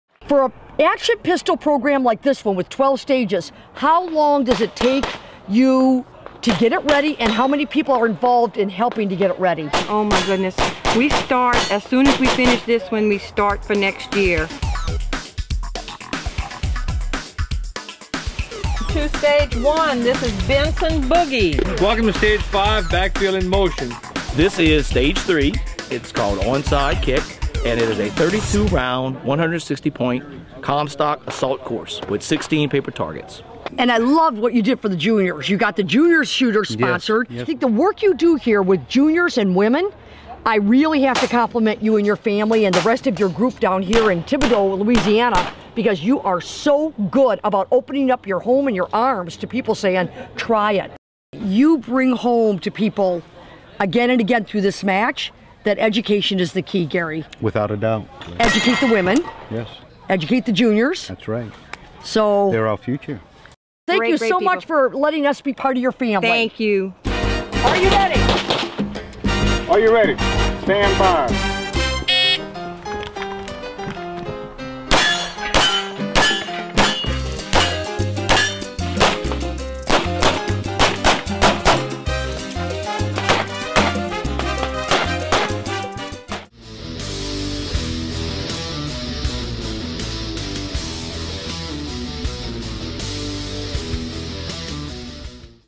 Gator Classic Shoot-Off
Here is a look at a family run shoot named the "Gator Classic" that does more than just give a place for people to shoot. It is helping youth get into the sport as well as providing great prizes to get more attendance. The "Babes With Bullets" crew is trying their best at this shoot.